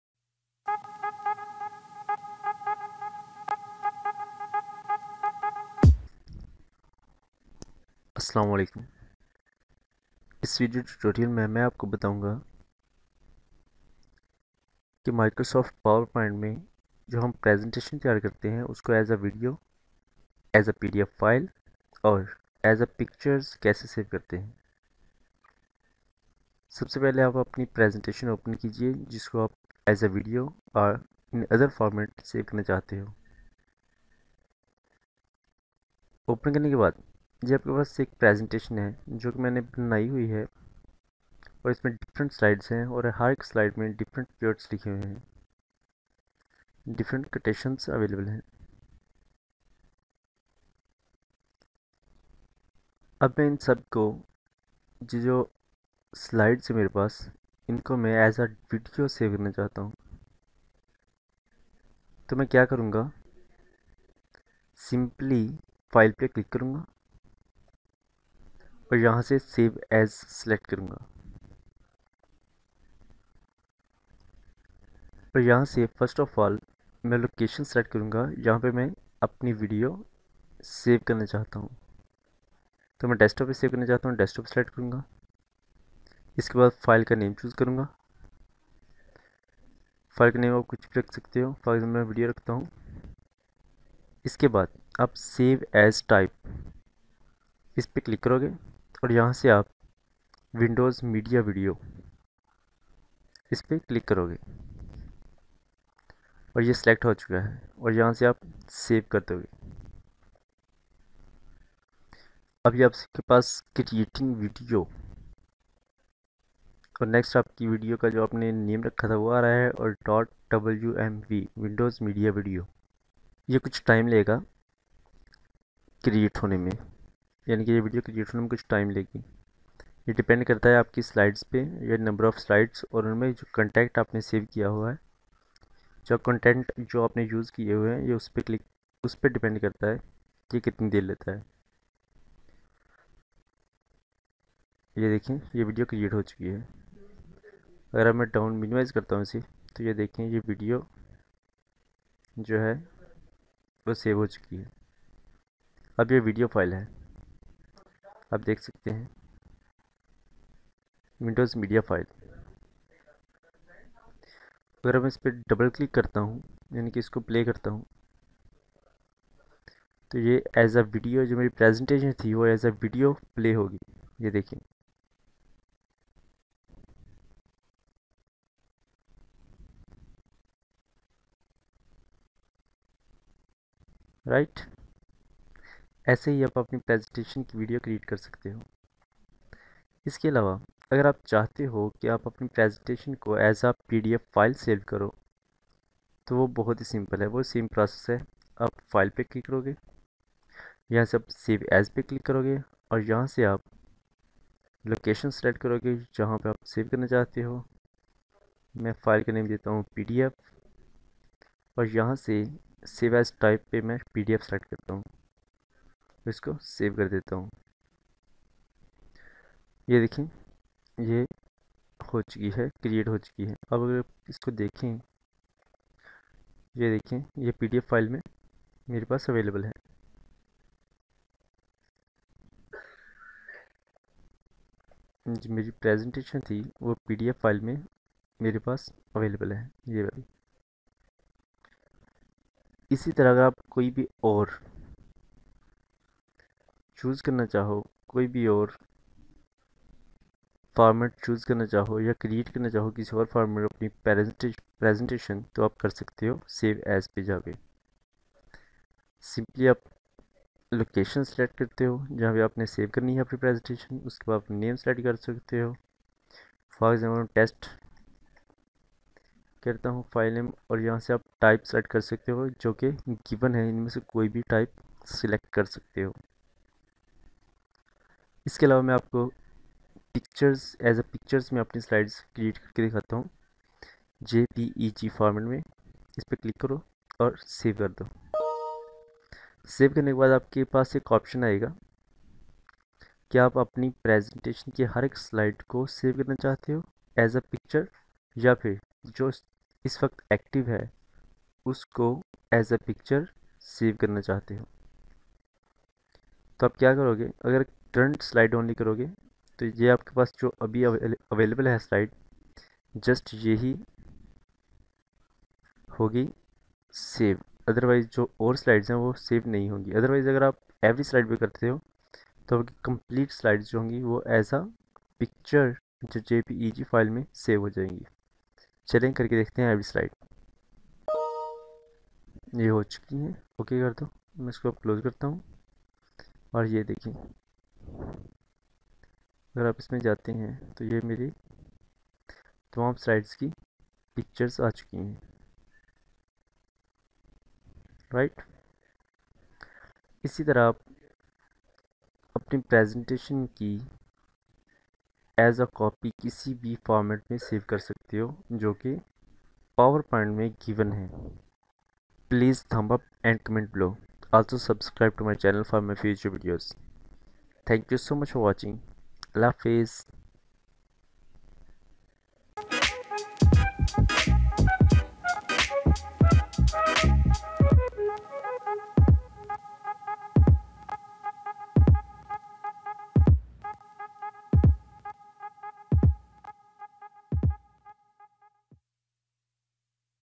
How To Save PowerPoint File In Video, PDF or Pictures
In this video Tutorial You Will Learn How To Save MS PowerPoint Presentation in Video, PDF or Image/Picture files. It’s useful when you create an animation for your videos and make that one in video file.